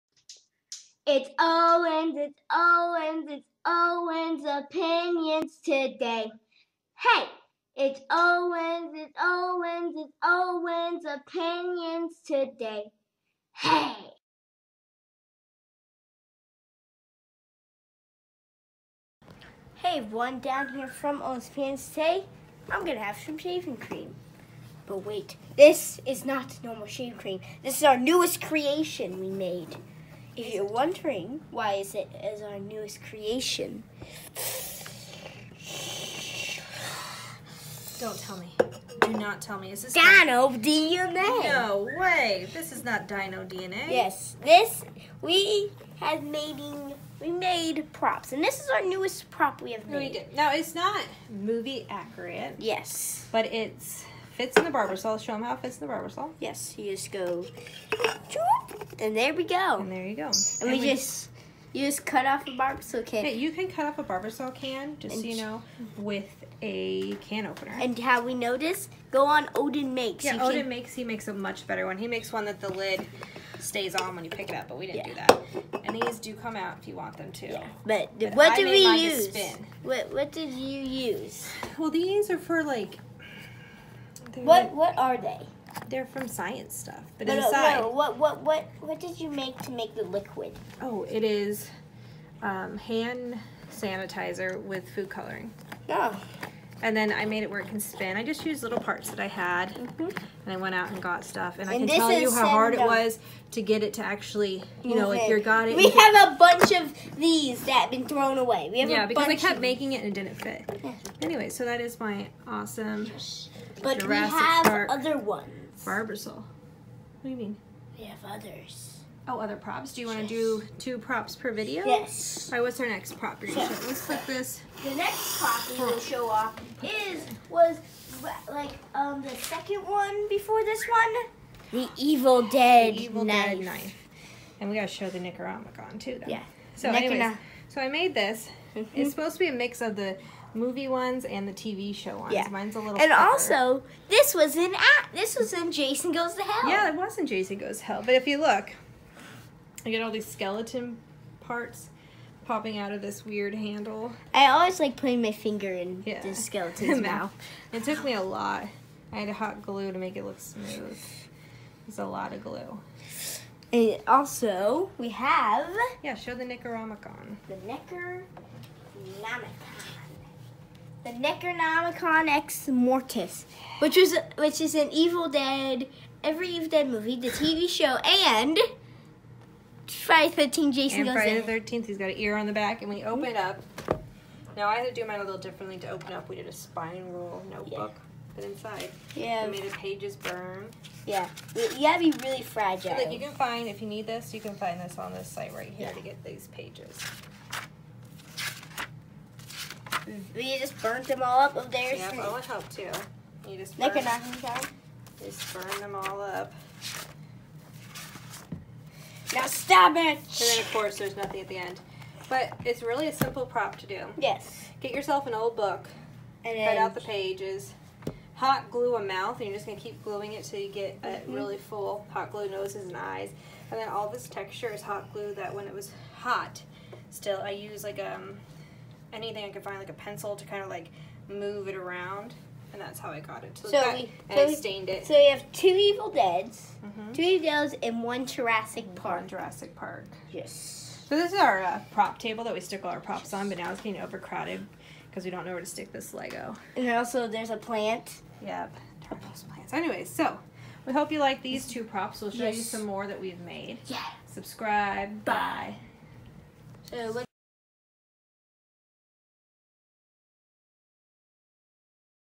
MY DIY probs Jurassic park and evil dead
my mom is good at making probs from movies i love so here are some of the ones she has made